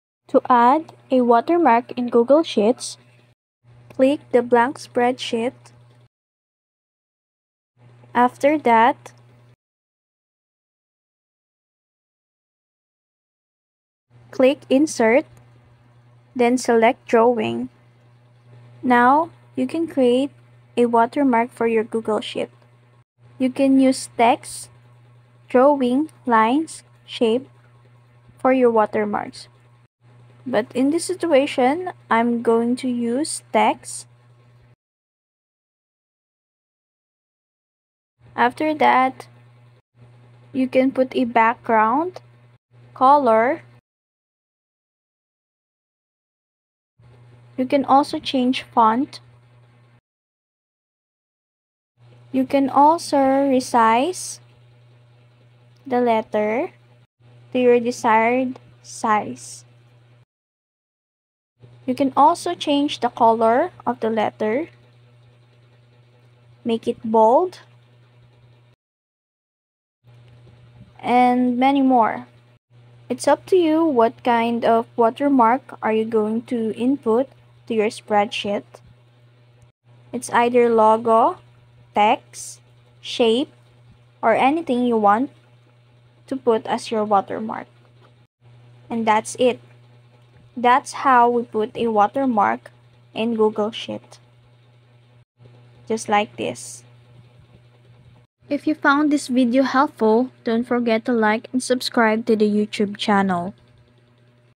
How to Add Watermark in Google Sheets
In this video, I'll show you how to add a watermark to Google Sheets. The method is very simple and clearly described in the video. By following all the steps in the video, you'll be able to easily learn how to add a watermark to a sheet in Google Sheets.